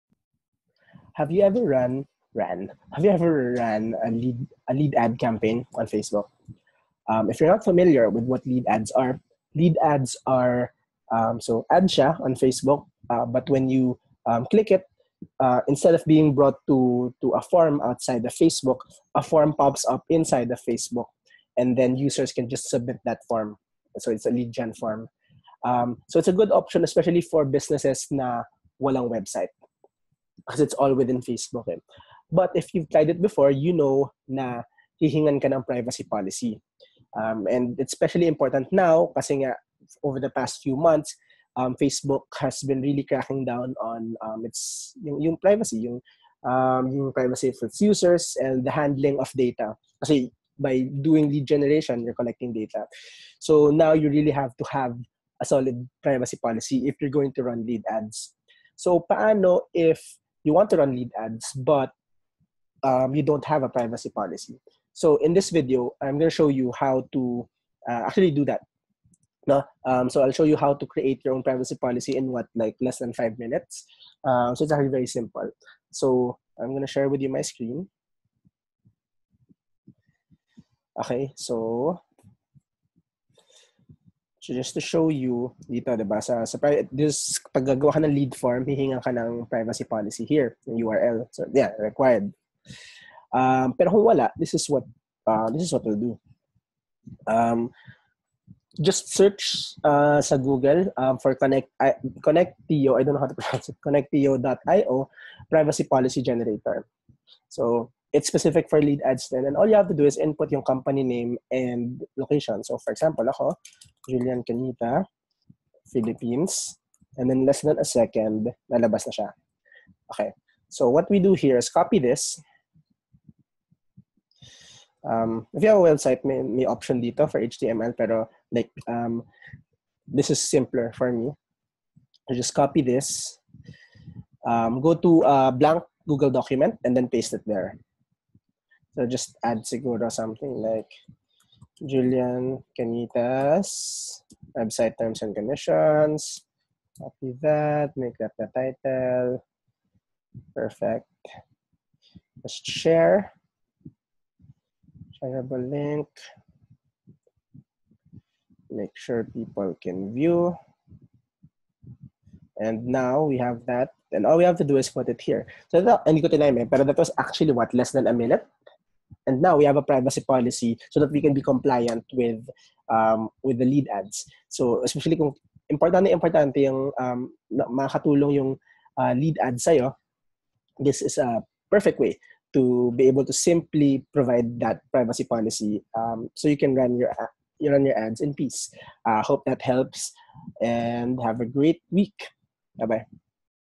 How To Create A Privacy Policy for FB Lead Ads
In this video, we'll discuss how to quickly create a privacy policy for your FB Lead Ads if you don't have a website or you don't already have a privacy policy